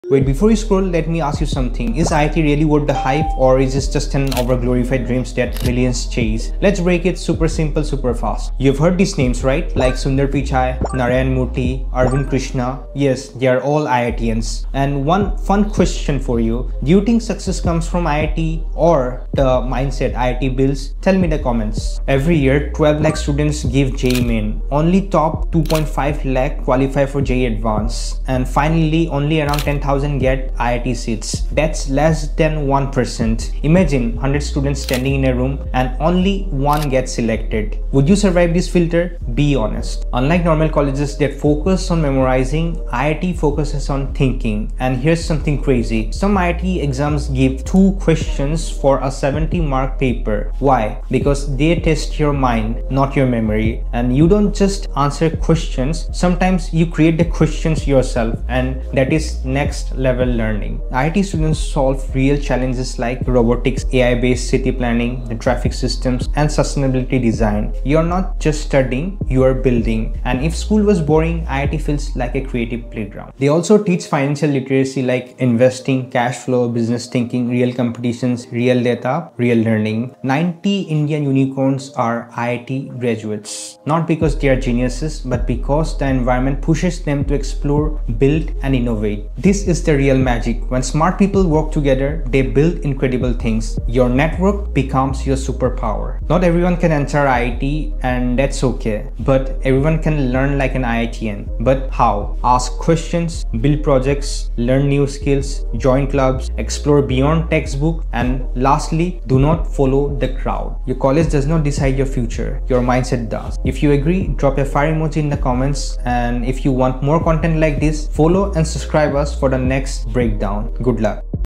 How IIT Education Creates India’s Best Billion-Dollar Founders
Think cracking IIT JEE is just about studying hard? Think again. This video breaks down the brutal selection filter, the 0.8% acceptance rate, and reveals why the entrance exam is one of the toughest in the world. Understand the entire admission process from Mains to Advanced and why it's a game of the absolute best minds.